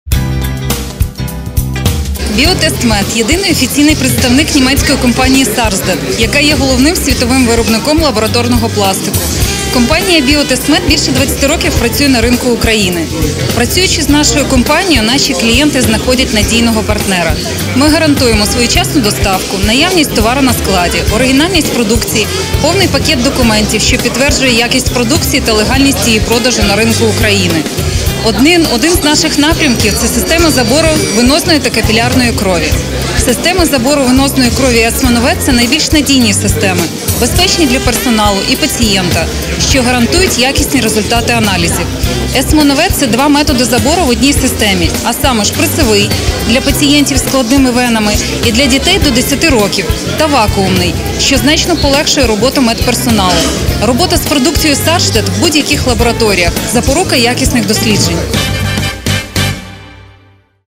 ТОВ " Біо Тест Мед"
ТОВ " Біо Тест Мед" - єдиний офіційний представник німецької компані "Sarsted" на виставці "Гал-Мед 2018" у Львові